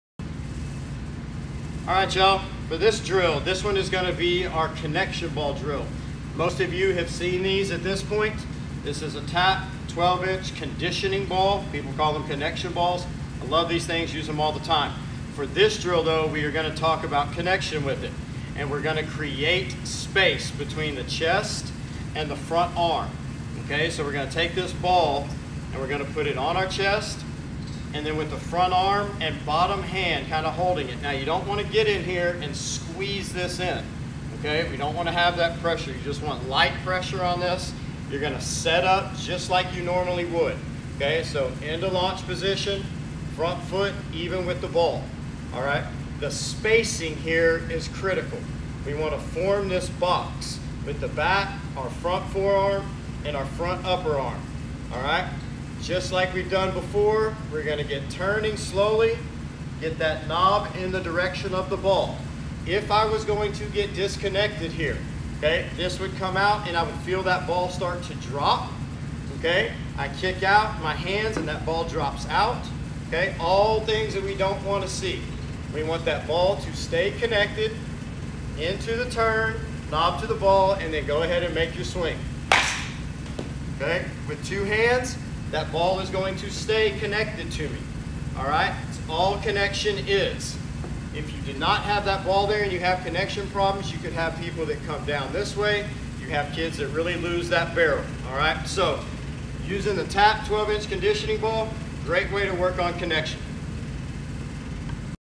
Connection Ball - short swing drill | Hitting Done Right
This drill helps players who get disconnected (long) in their swing. The use of the connection ball enhances the awareness of the upper body specifically arms as the player initiates the turn phase of the swing. The ball size can be inflated or deflated to fit each player.